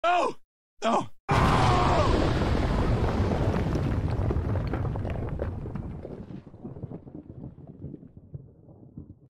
Bomber Sounds
Sounds of yelling and a bomb